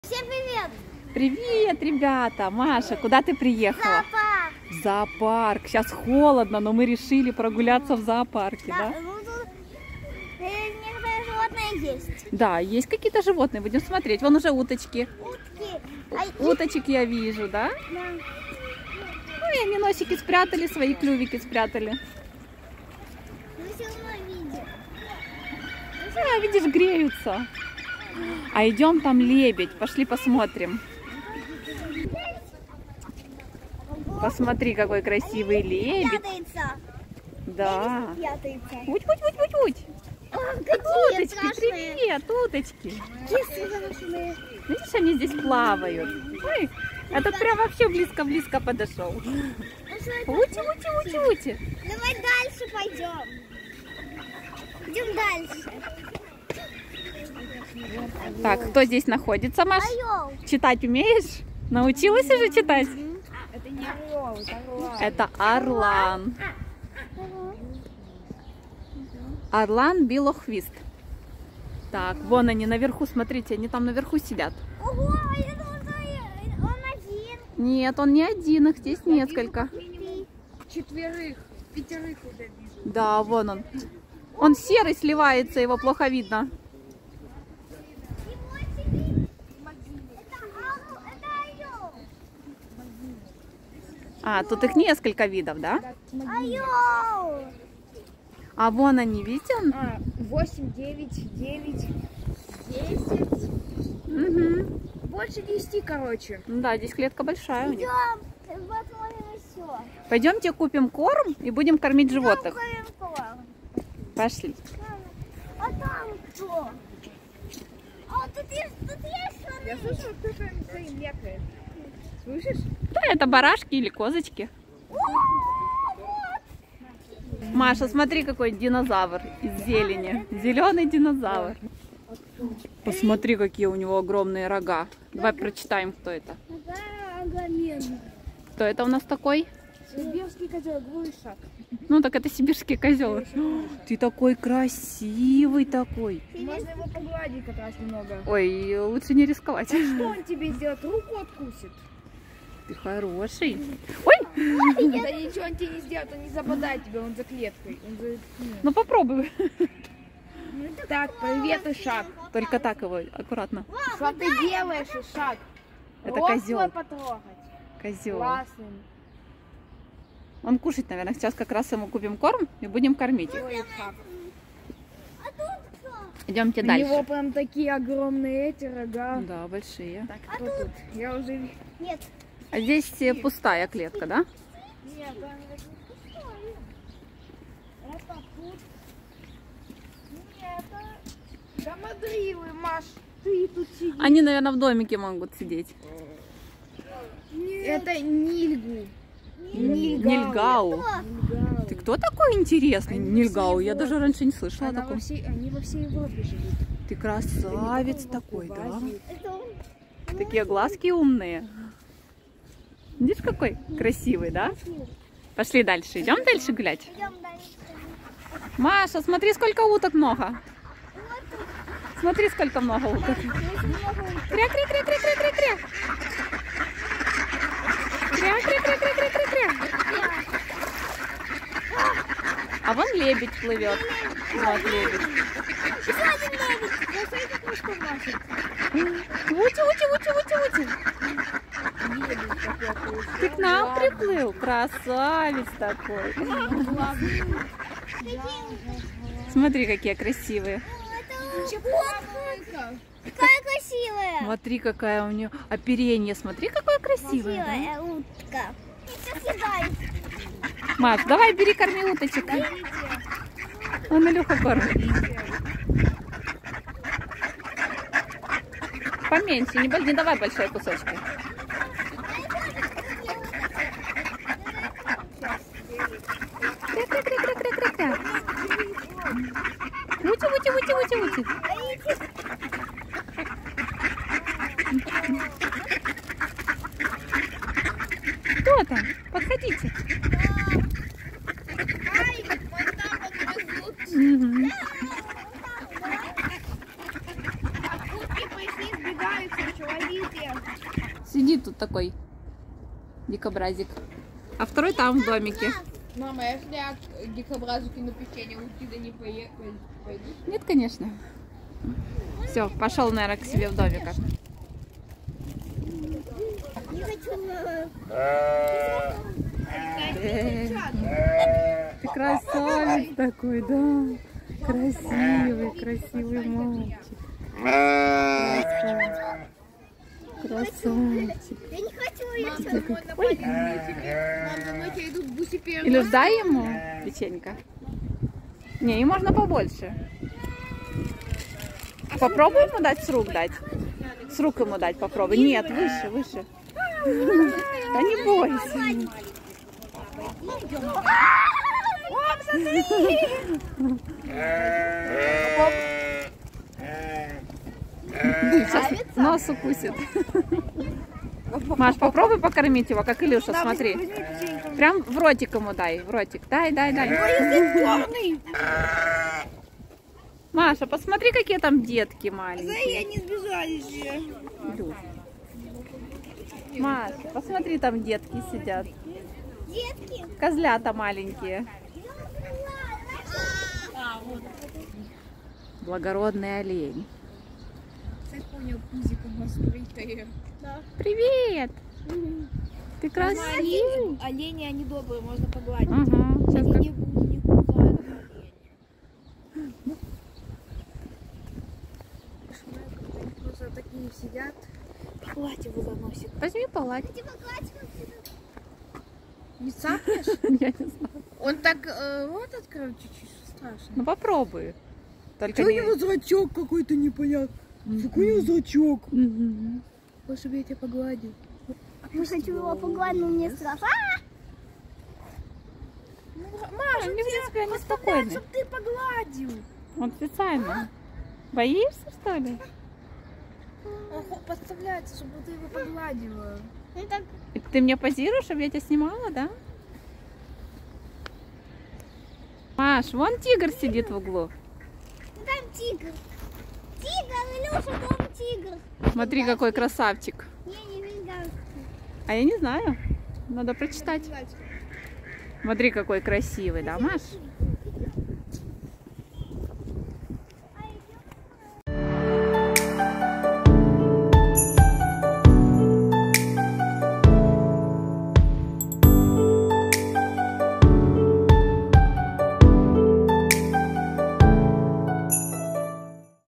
Прогулка в зоопарке
Прогулка в зимнем зоопарке в Одессе